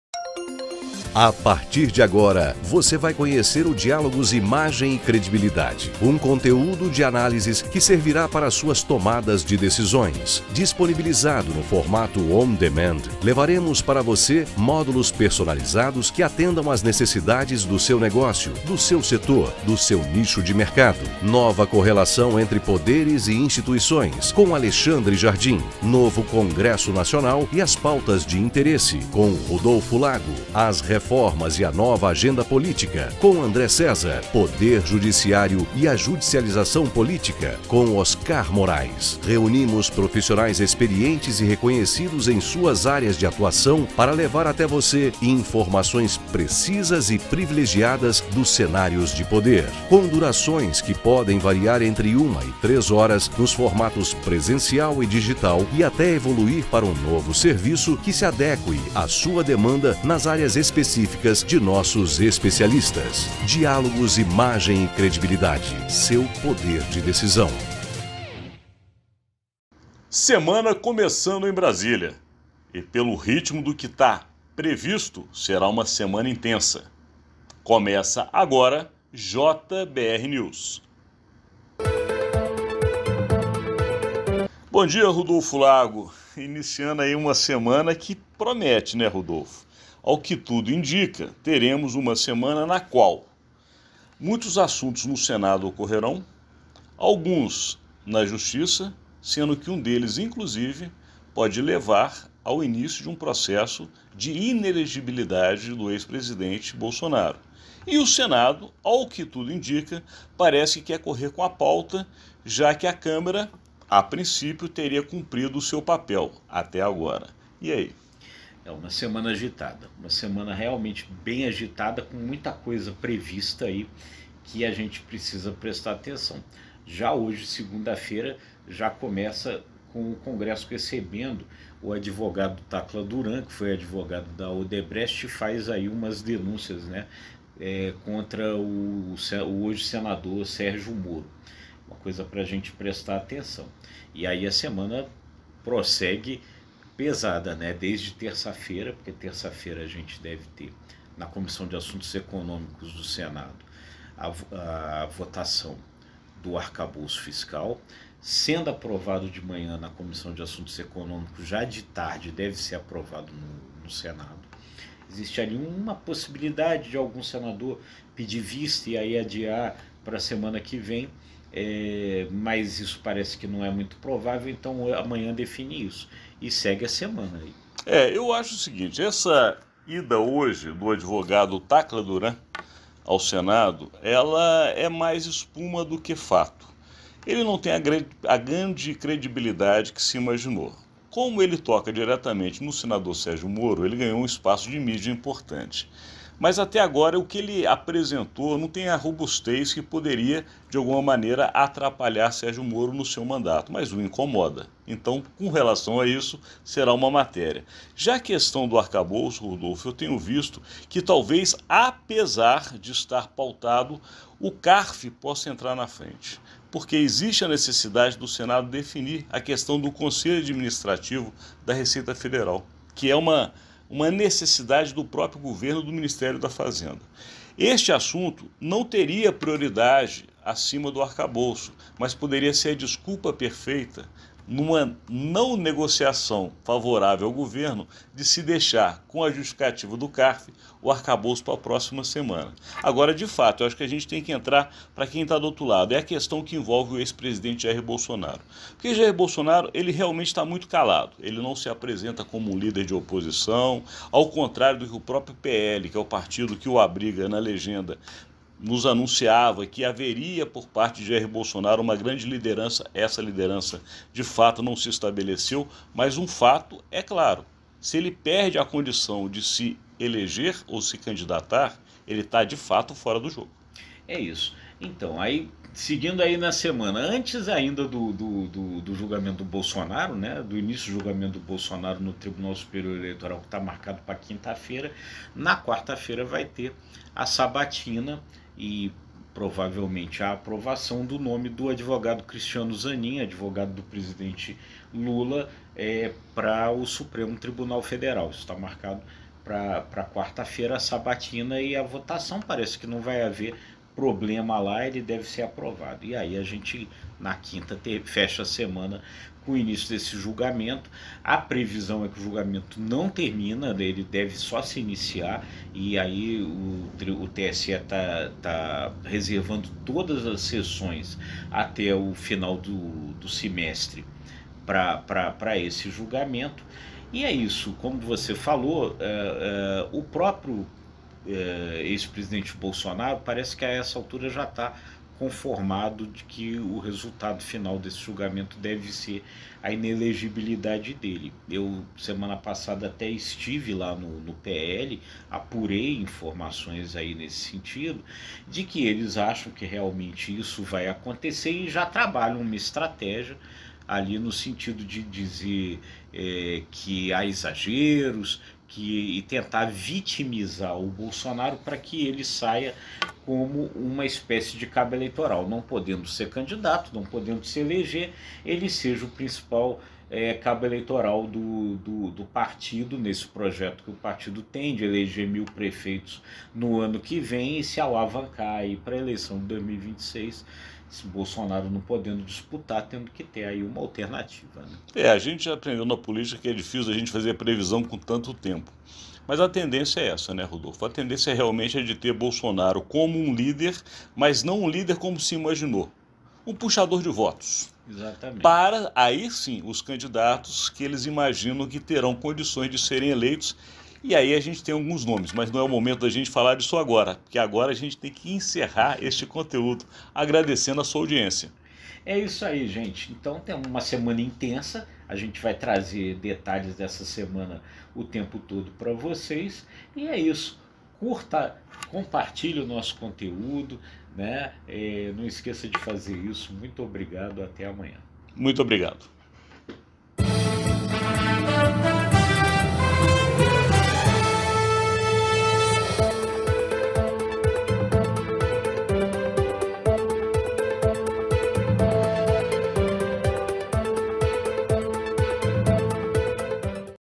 JBr News 116: Do arcabouço ao julgamento de Bolsonaro, a semana promete
A semana termina com as festas de São João. Mas começa com muita atividade na Esplanada dos Ministérios. Enquanto o presidente Luiz Inácio Lula da Silva embarca para nova viagem à Europa, em Brasília há muita coisa prometida na agenda. A começar pelo depoimento do advogado Tacla Duran no Congresso. Na terça-feira, a votação do arcabouço fiscal. Na quarta, a sabatina e provável aprovação do advogado de Lula Cristiano Zanin para o Supremo Tribunal Federal (STF). E, finalmente, na quinta-feira, o início do julgamento do Tribuna. Superior Eleitoral (TSE) que poderá tornar o ex-presidente Jair Bolsonaro inelegível. Cada um dos pontos dessa extensa agenda recebe aqui o comentário de Alexandre Jardim e Rudolfo Lago no JBrNews de hoje.